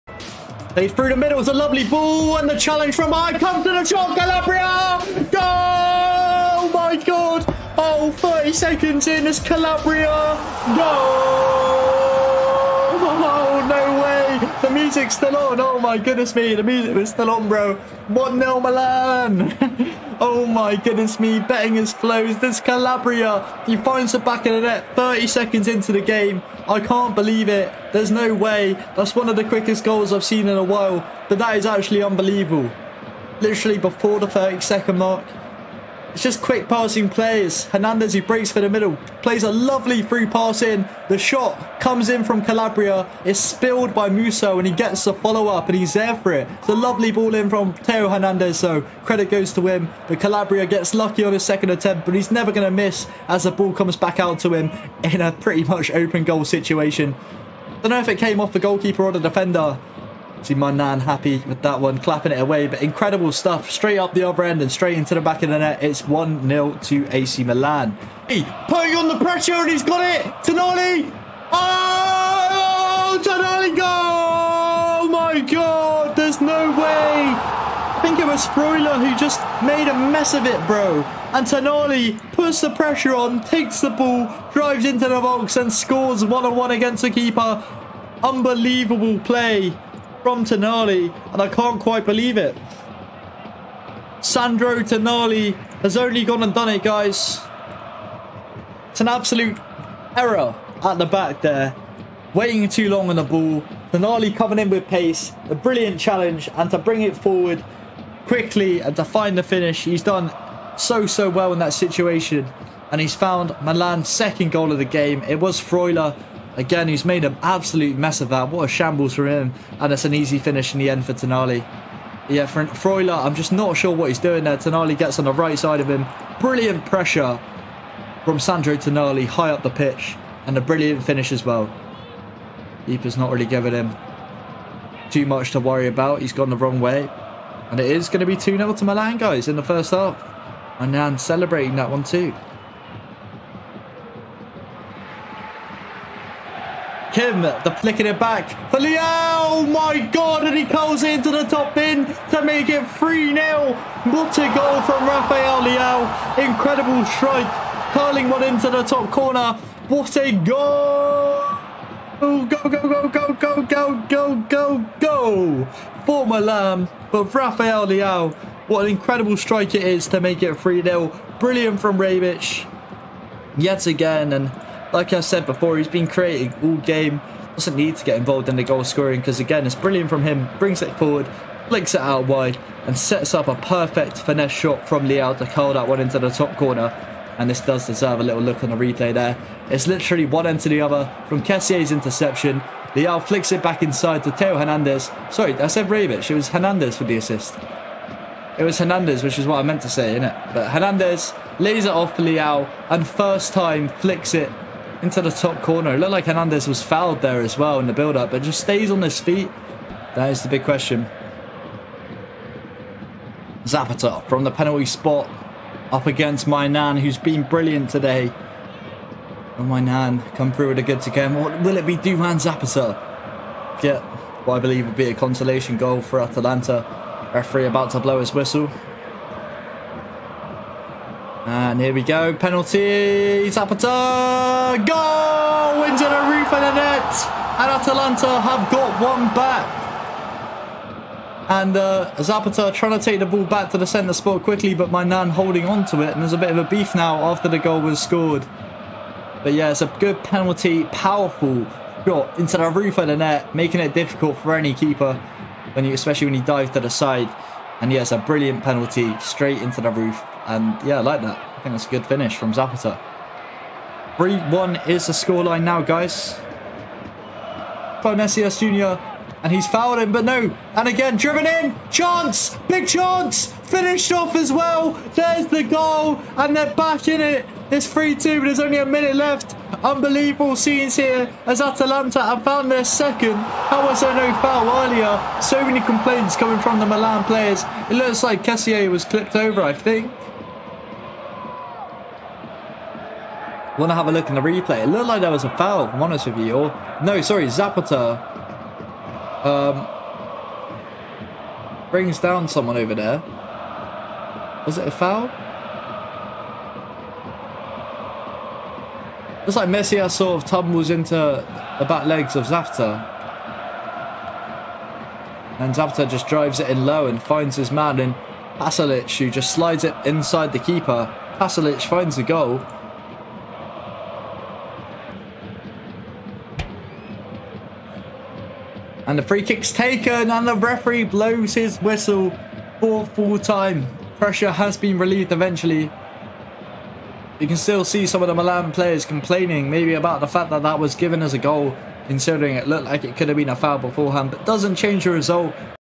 ATALANTA vs AC MILAN Highlights (With Jamzor)
Highlights From ATALANTA Vs AC Milan with Jamzor
All Footage Comes From The Game Played On The 3RD October 2021
All The Footage IN The Video Comes From Jamzor Live Football